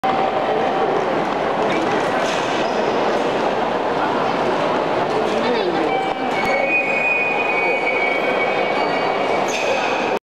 東京駅丸の内改札時計
東京/中央区にある日本首都駅です
ドーム改札にあります